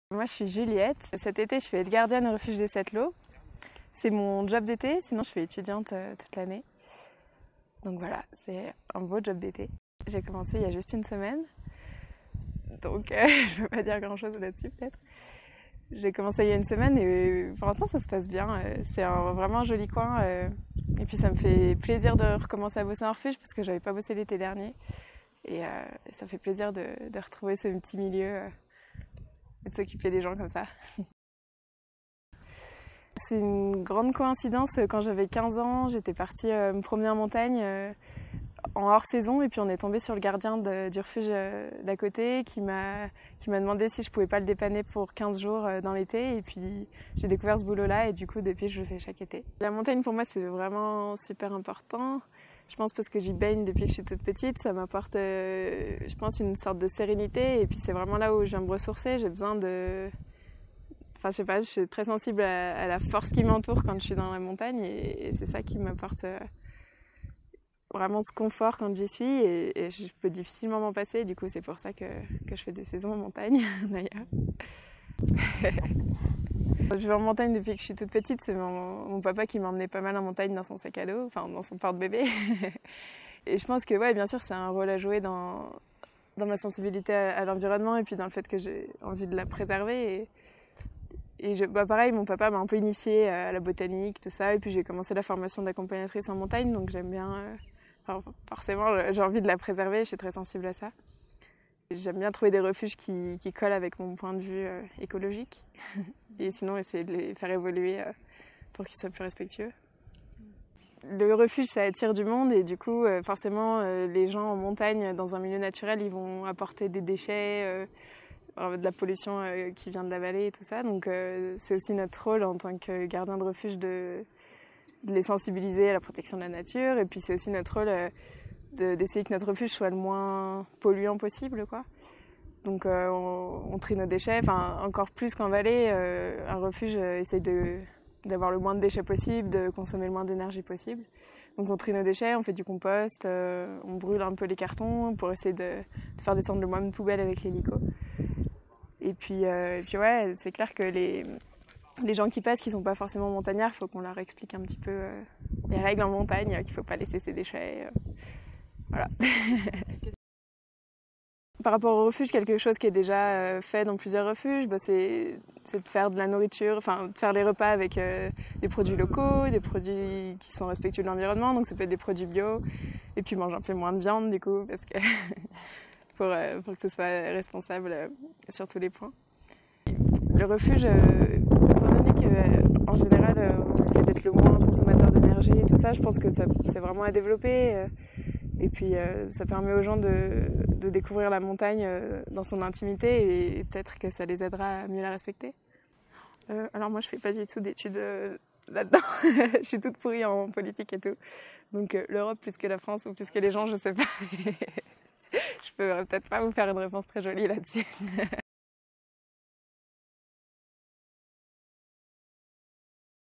Juliette - Aide Gardienne au Refuge des 7 Laux en Belledonne
Interview de Juliette, aide-gardienne au refuge des Sept Laux
Vidéo réalisée lors de l'écotraversée en Belledonne 2018
dans le cadre du projet Leaving Labs Erasmus+ avec l'association Cipra France
Pour soutenir cette réalisation et les prochaines, n'hésitez pas à faire un don :